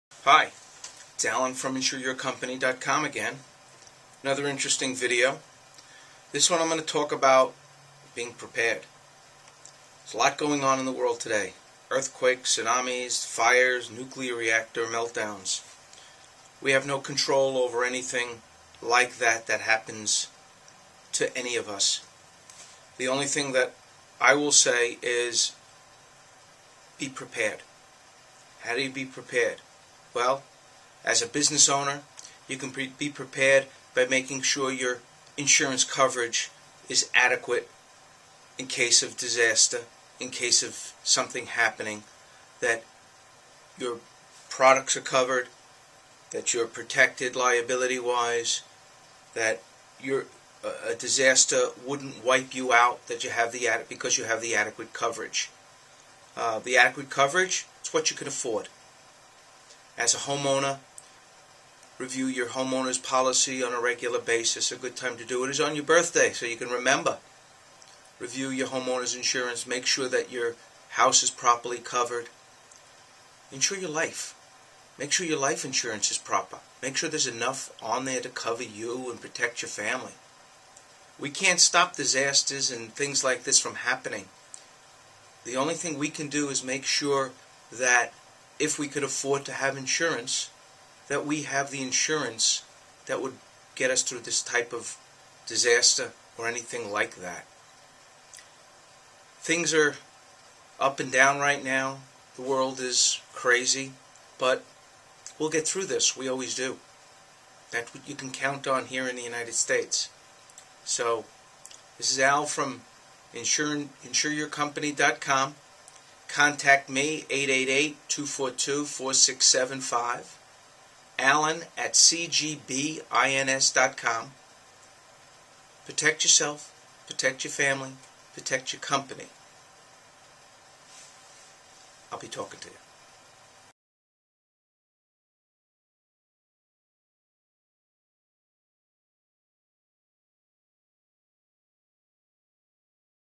episode 21 Protect Yourself
Alan give you some advice about the uncertain times.